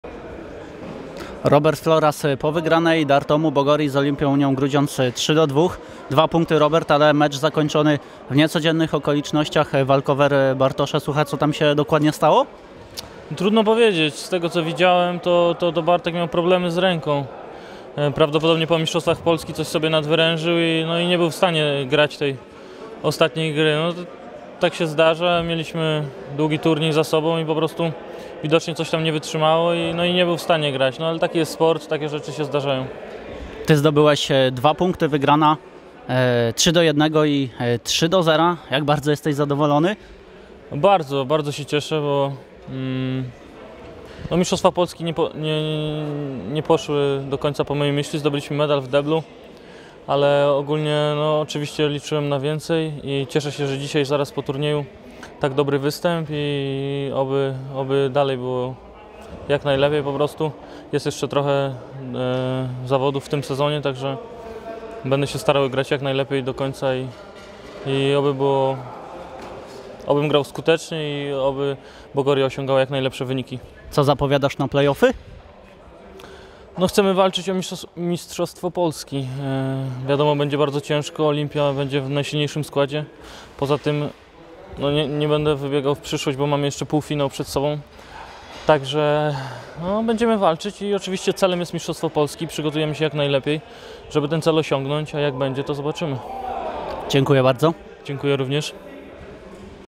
Wywiad: Robert Floras po wygranej 3:2 z Olimpią-Unią Grudziądz
Spotkanie Wschodzący Białystok Superligi Tenisa Stołowego